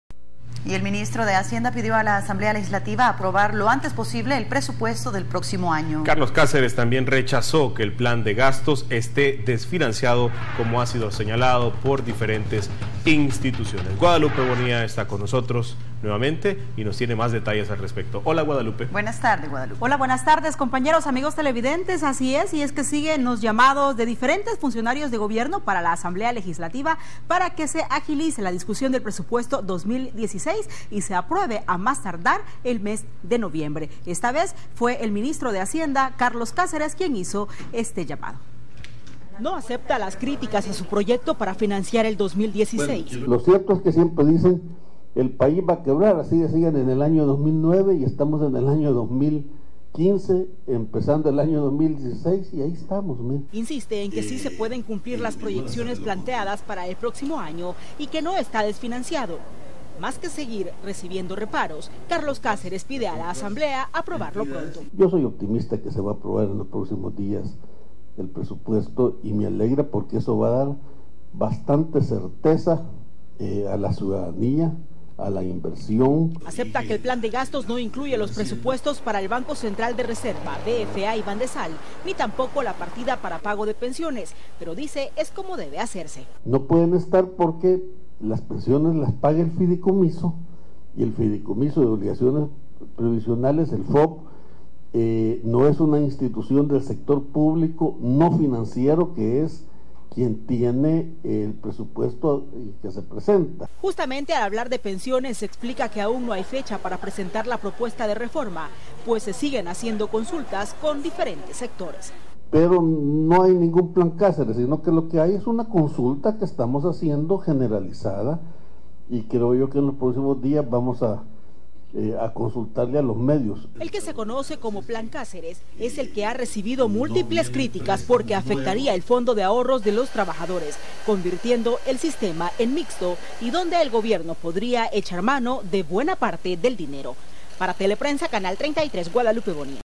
MINISTRO DE HACIENDA PIDE APROBACIÓN DEL PRESUPUESTO 2016
El ministro de hacienda pidió a la asamblea legislativa aprobar lo antes posible el presupuesto del próximo año.
Carlos Cáceres también rechazo que el plan de gastos este desfinanciado.